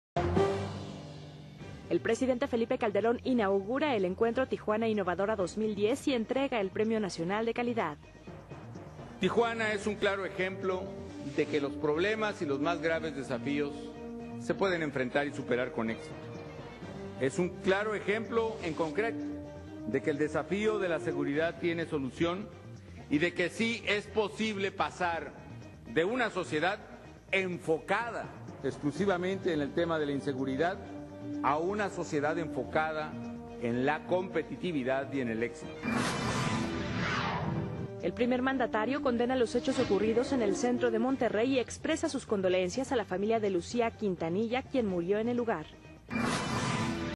INAUGURACIÓN DEL PRIMER ENCUENTRO DE 'TIJUANA INNOVADORA 20010'
El dia de ayer se inauguró el primer encuentro de 'Tijuana Innovadora 2010", con el propósito de juntar la tecnología con la cultura.